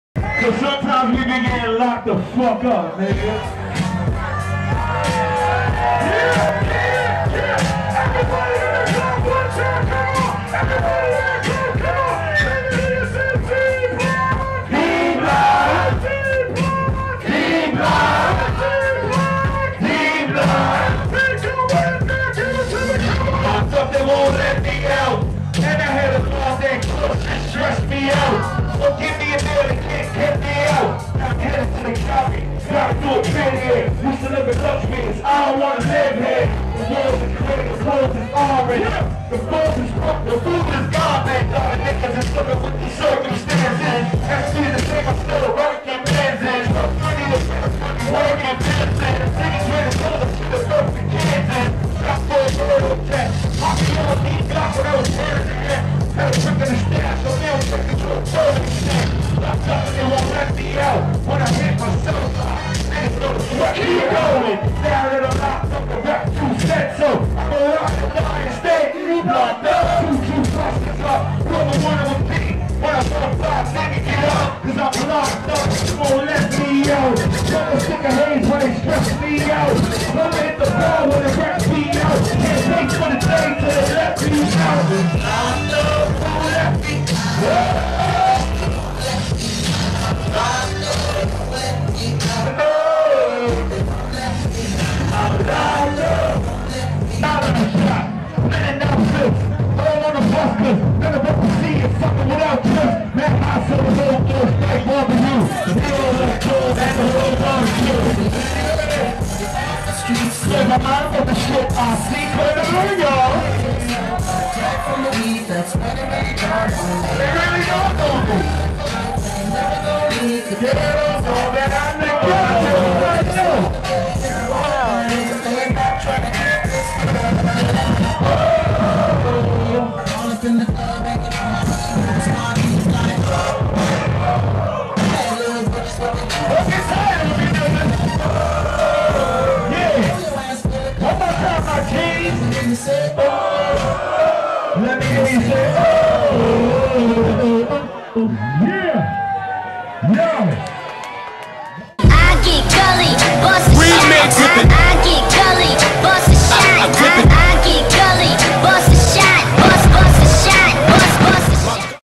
STYLES P LOCKED UP IN PITTSBURGH!!! {EXCLUSIVE}
D-BLOCK VETERAN STYLES P CAUGHT WILDN OUT ON FILM IN PITTSBURGH PA NIGHTCLUB